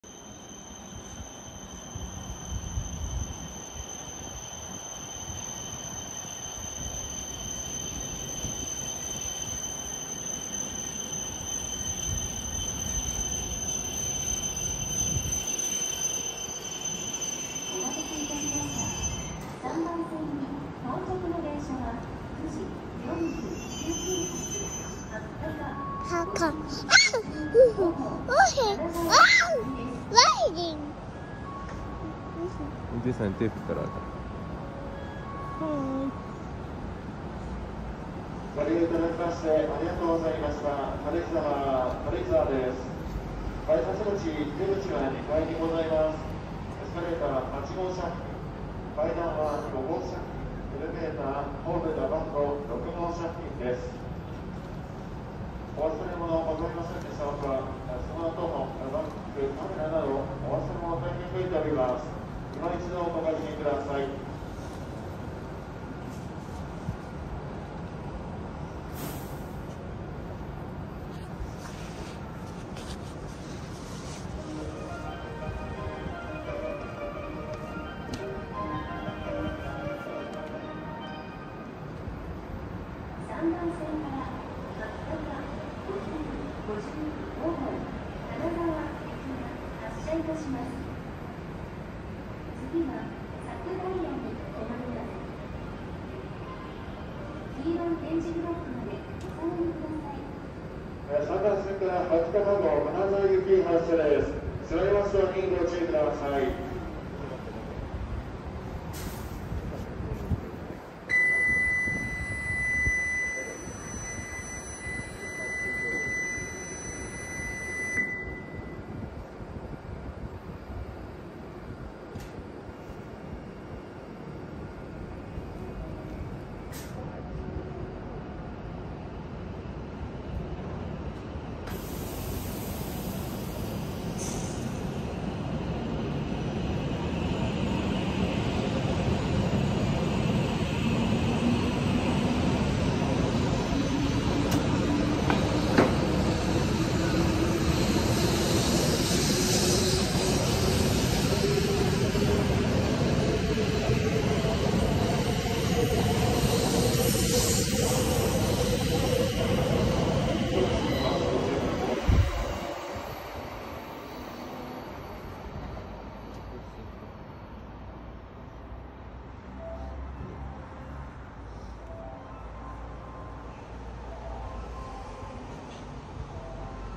Hokuriku shinkansen series W7 at karuizawa station
The blue bullet train running in the snow is beautiful.बर्फ में दौड़ती नीली बुलेट ट्रेन बेहद खूबसूरत है।